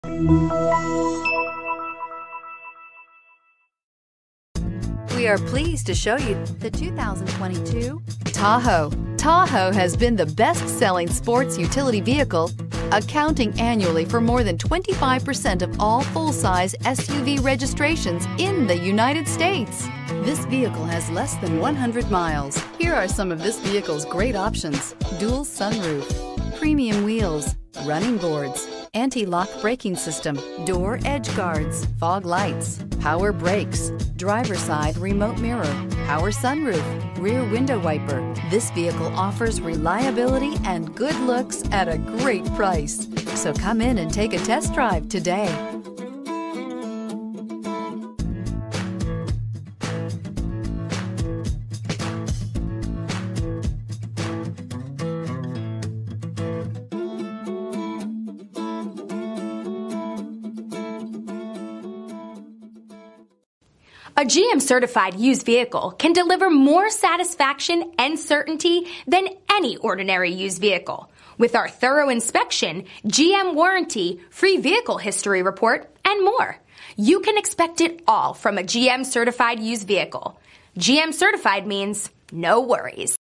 2022 Chevrolet Tahoe P11778
2022 Chevrolet Tahoe Z71

It also includes Roadside Assistance, Courtesy Transportation.  This vehicle also comes with 12,000 warranty miles and has been subjected to a rigorous, 172 -point inspection for condition and appearance.  For a good-looking vehicle from the inside out, this suv 4wd features a classic red exterior along with a black interior.  Youve never felt anything more comfortable than leather seats.  The woodgrain trim on this vehicle adds a luxurious feel.  Keep safe and comfortable with the telescoping steering wheel.  Easily lock and unlock your doors with a simple hands-free click thanks to keyless entry.  Open the sunroof on this vehicle and get your daily Vitamin D!